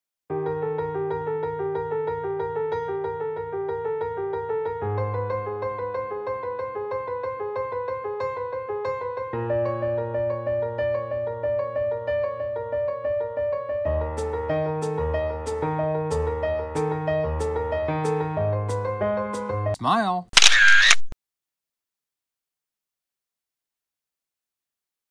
New Student Orientation Group Photo
The students attending the New Student Orientation event at Heritage University pose for a group picture. Here's a video of the photo session.